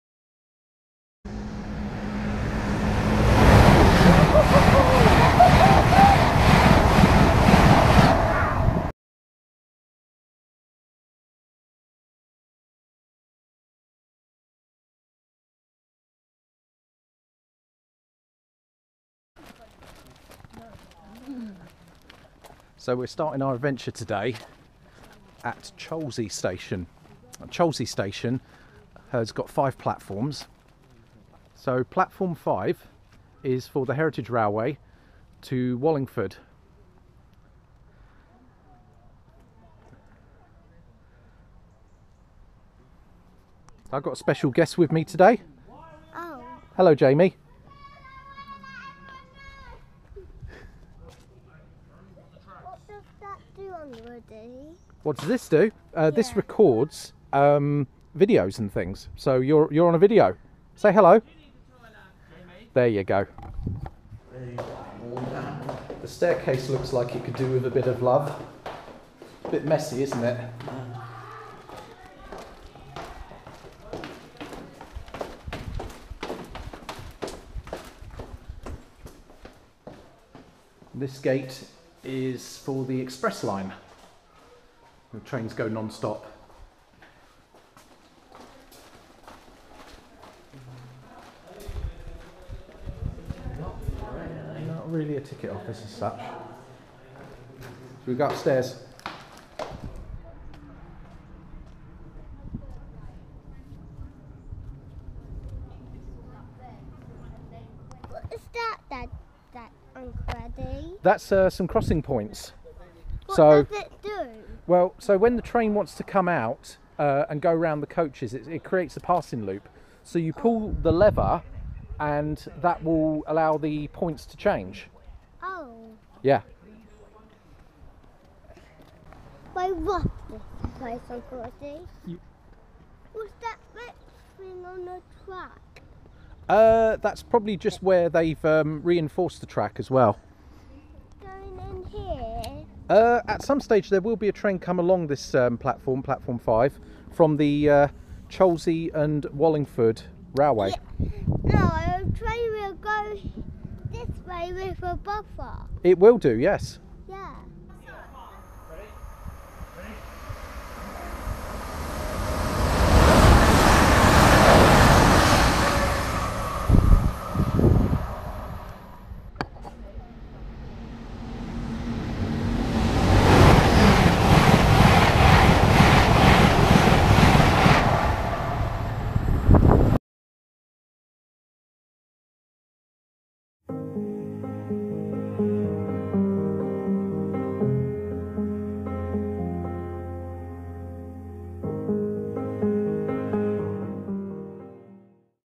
Cholsey Railway Station Visit & A Special Guest
As part of our visit to Didcot, we dropped off the cars at Cholsey and took the one stop to Didcot Parkway. My special guest is my nephew, Jamie